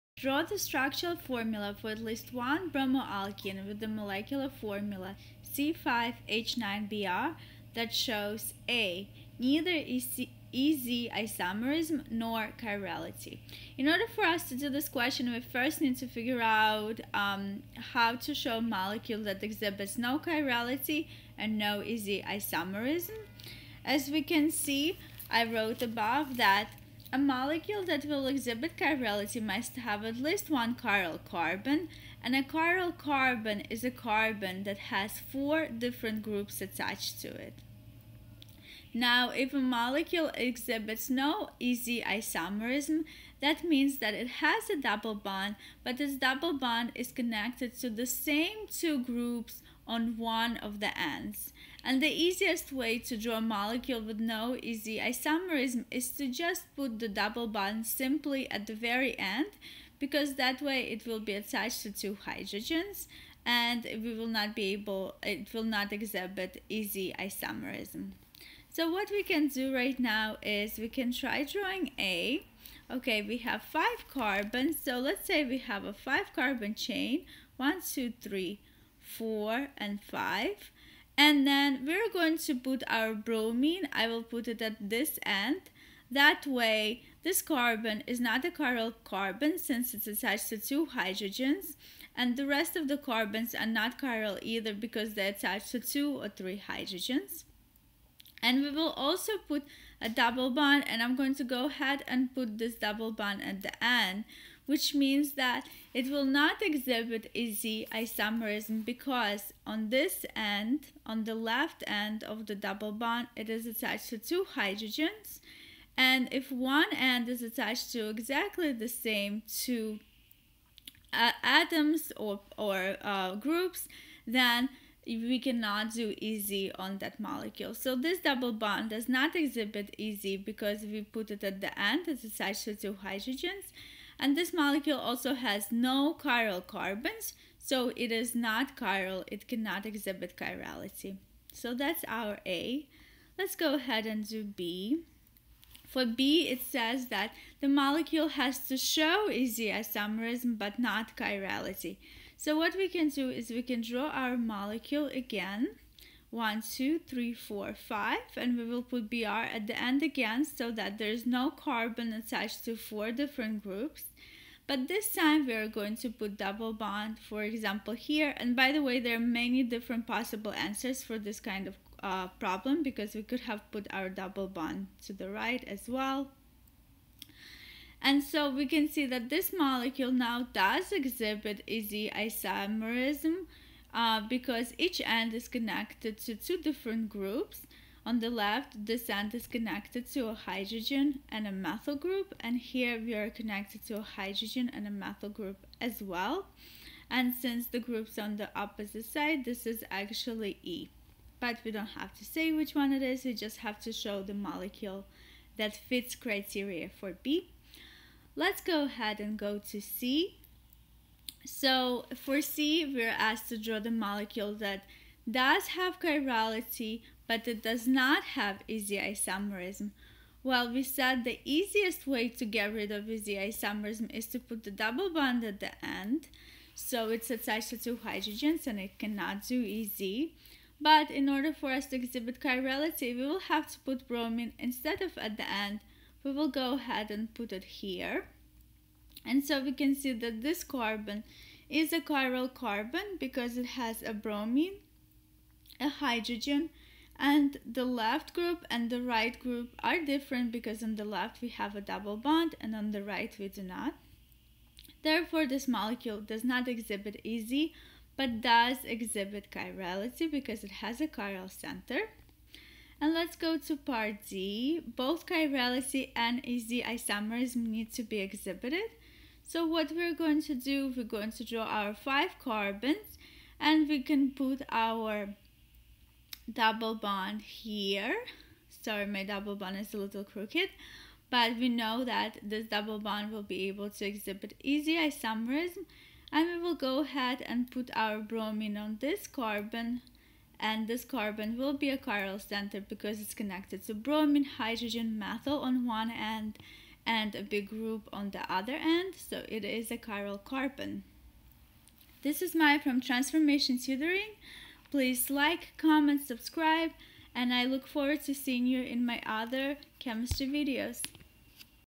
Draw the structural formula for at least one bromoalkene with the molecular formula C5H9Br
Draw the structural formula for at least one bromoalkene with the molecular formula C5H9Br that shows:
(a) Neither E,Z isomerism nor chirality.
(b) E,Z isomerism but not chirality.
(d) Both chirality and E,Z isomerism.
In order to do this questions we must know two things. A molecule that does not have a chiral center will not be chiral. A chiral center is a carbon that is attached to four different groups. Therefore, if we need to draw a molecules with no chirality, we need to make sure there are no carbons with four different groups.

The second concept we need to understand is that a molecule with no E/Z will have a double bond connected to the same two groups on one side. It is easiest to just put a double bond at the end of the molecule so that it is attached to two hydrogens on one end and can not be E/Z